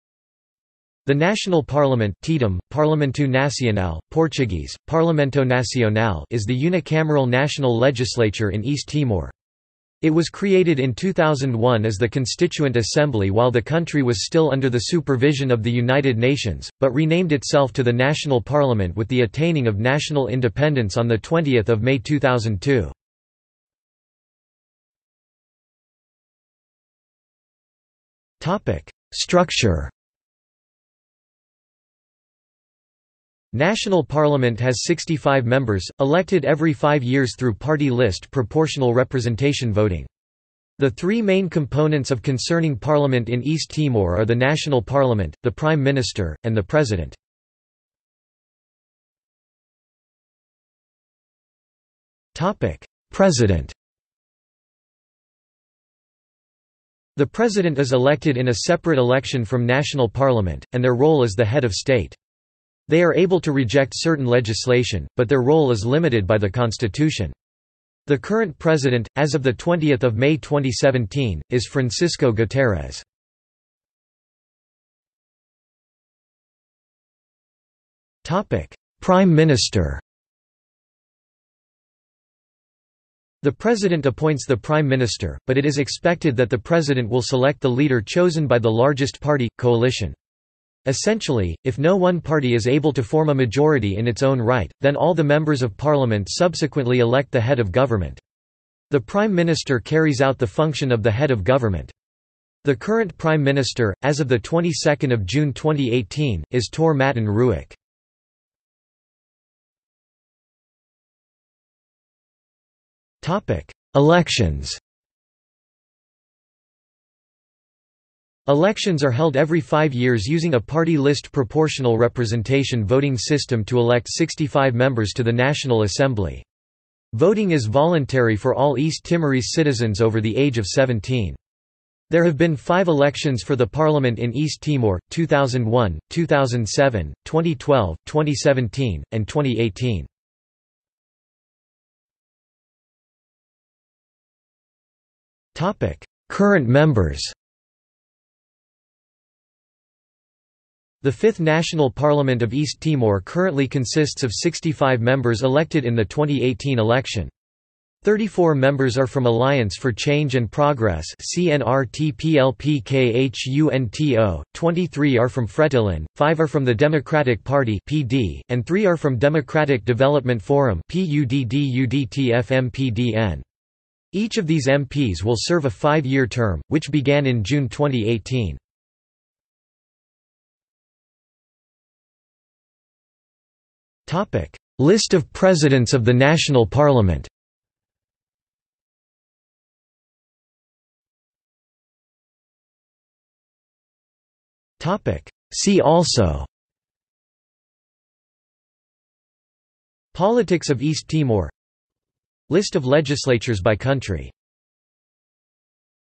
National Parliament (East Timor) | Wikipedia audio article
This is an audio version of the Wikipedia Article:
National Parliament (East Timor)

00:00:24 1 Structure
00:00:49 1.1 President
00:01:14 1.2 Prime Minister
00:01:49 2 Elections
00:02:21 3 Current members
00:03:00 4 List of Presidents of the National Parliament
00:03:11 5 See also

- Socrates

SUMMARY
The National Parliament (Tetum: Parlamentu Nasionál, Portuguese: Parlamento Nacional) is the unicameral national legislature in East Timor. It was created in 2001 as the Constituent Assembly while the country was still under the supervision of the United Nations, but renamed itself to the National Parliament with the attaining of national independence on 20 May 2002.